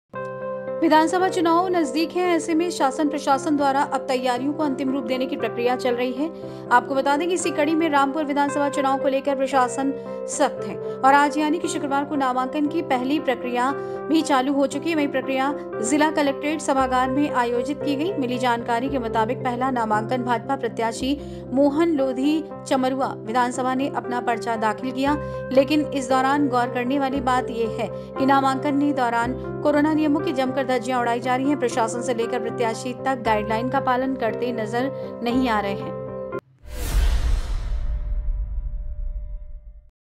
भाजपा प्रत्याशी मोहन कुमार लोधी ने किया नामांकन ,,,यूं उड़ी कोरोना नियमों की धज्जियां
विधानसभा चुनाव नजदीक है। ऐसे में शासन प्रशासन द्वारा अब तैयारियों को अंतिम रूप देने की प्रक्रिया चल रही है। आपको बता दे कि इसी कड़ी में रामपुर विधानसभा चुनाव को लेकर प्रशासन सख्त है और आज यानी की शुक्रवार को नामांकन की पहली प्रक्रिया भा चालू हो चुकी है। वही प्रक्रिया जिला कलेक्ट्रेट सभागार में आयोजित की गई. मिली जानकारी के मुताबिक पहला नामांकन भाजपा प्रत्याशी मोहन लोधी चमरूआ विधानसभा ने अपना पर्चा दाखिल किया. लेकिन इस दौरान गौर करने वाली बात ये है, कि नामांक ने दौरान कोरोना नियमों की जमकर धज्जियां उड़ाई जा रही है। प्रशासन से लेकर प्रत्याशी तक गाईड लाइन का पालन करते नजर नही आ रहे